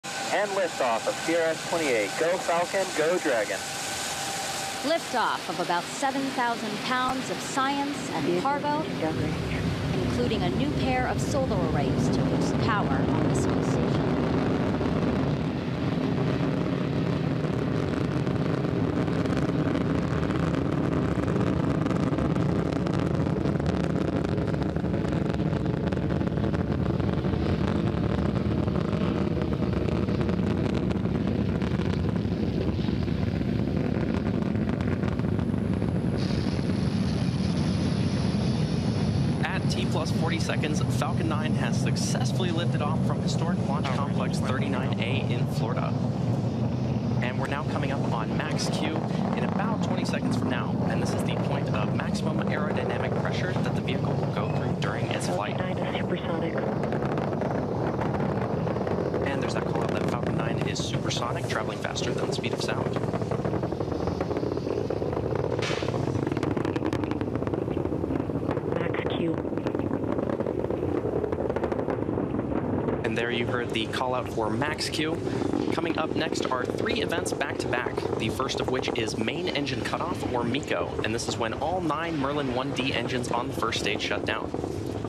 SpaceX Launches Resupply Mission to Space Station
SpaceX launched the Falcon 9 rocket on a mission to resupply the International Space Station. The booster rocket safely landed back on Earth a few minutes later. This is the 39th flight for SpaceX already this year.